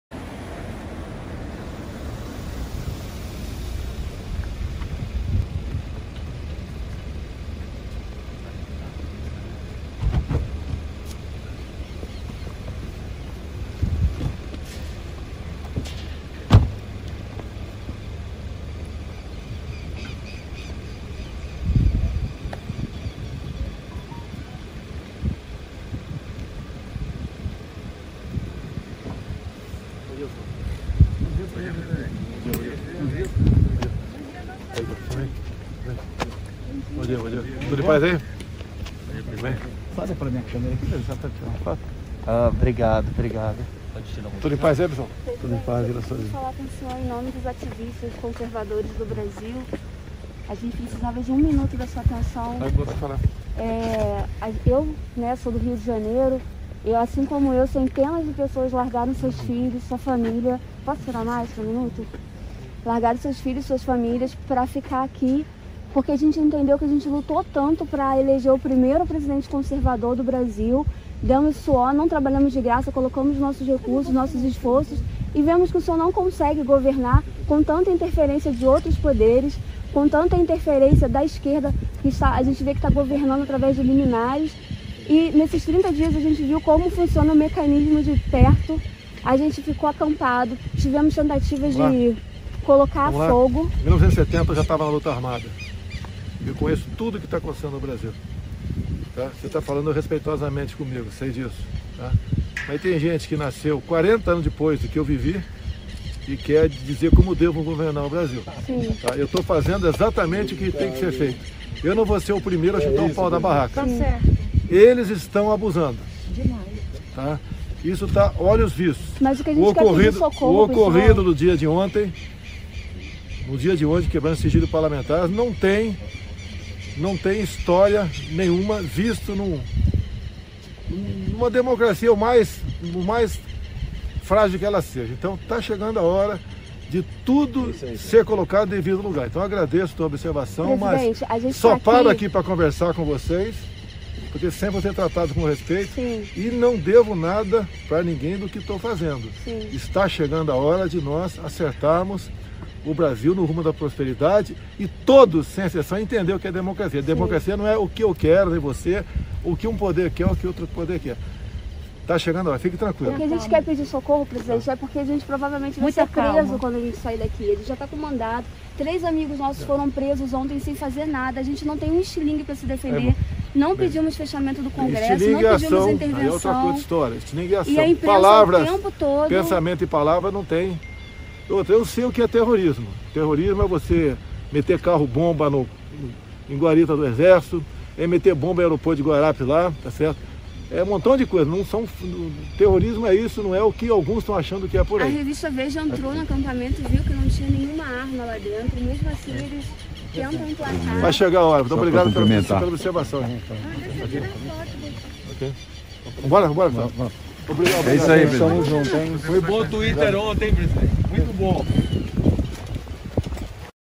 O presidente B0LSONAR0 está ciente dos abusos autoritários .
Presidente Bolsonaro responde sobre pedidos de ajuda sobre abusos do STF.